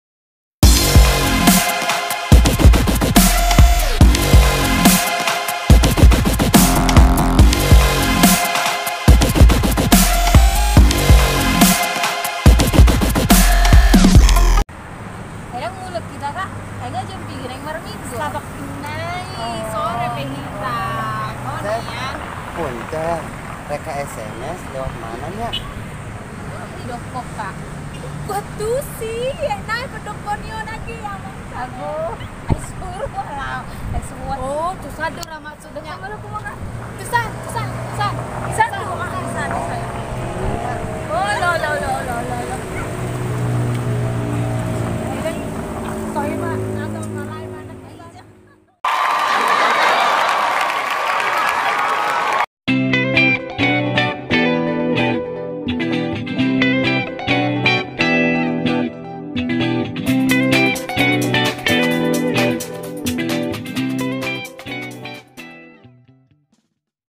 Lawak Batak Terbaru SIMENTEL-MENTEL 2019
Disuatu pagi yang cerah mereka duduk berdua lagi asik bercerita sambil makan snack. Tiba-tiba datang seorang pemuda yang mau menanyakan alamat. Karena mereka tidak tau apa yang dibicarakan pemuda tersebut mereka asal jawab aja DISAN/DISANA sambil menunjukkan arah karena mereka hanya tau SMS(Summarecon Mall Serpong).
Hahahaha😄😄😄